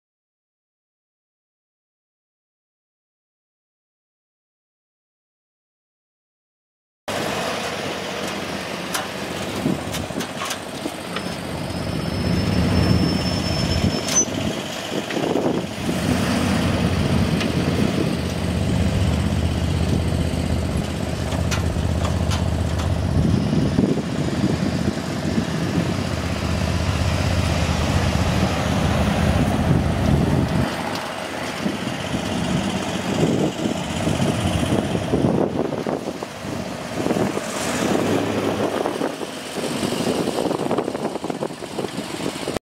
2008 Chevrolet Silverado 3500HD utility bed pickup truck | no-reserve auction December 14, 2017
View a detailed listing of this item and place bids

2008 Chevrolet Silverado 3500HD utility bed pickup truck
145,277 miles on odometer
6.0L V8 OHV 16V gas engine
Automatic transmission
AC and heat
Tilt
Cruise
AM/FM/CD
Manual windows and locks
Vinyl interior
Front tow hooks
Knapheide 6963 utility bed
8'2"L x 78"W
Receiver hitch
Trailer wiring
265/75R16 tires
1GBHC34K08E129138
It's located in Ada, OK, and we're selling it to the highest bidder regardless of price.